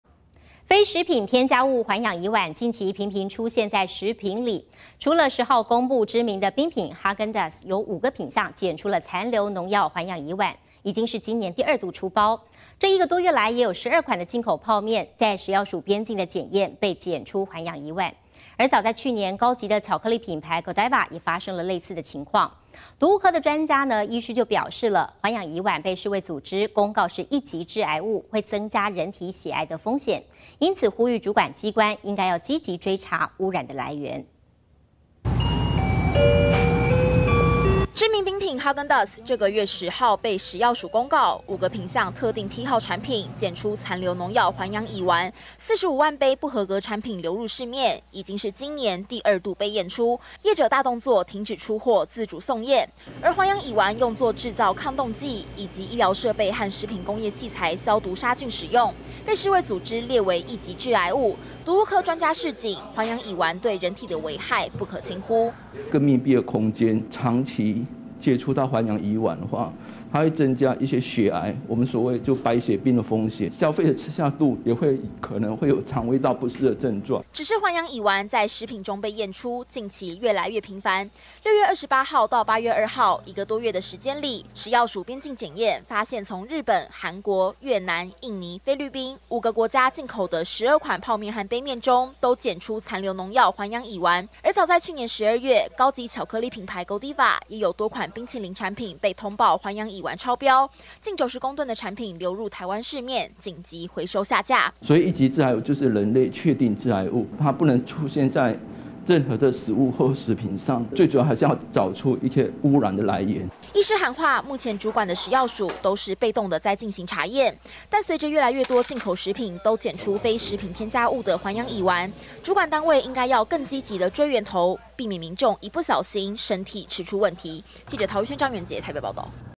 屬一級致癌物 環氧乙烷長期接觸增血癌風險
非食品添加物「環氧乙烷」，近期頻頻傳出在食品裡面檢出，除了10號公布知名冰品哈根達斯，有5個品項檢出殘留農藥環氧乙烷，已經是今年第2度出包；這一個多月來，也有12款進口泡麵，在食藥署邊境檢驗，被檢出環氧乙烷，而早在去年，高級巧克力品牌Godiva，也發生類似狀況。毒物科醫師表示，環氧乙烷被世衛組織公告是一級致癌物，會增加人體血癌風險，其危害不可輕忽，因此呼籲主管機關應積極追查污染來源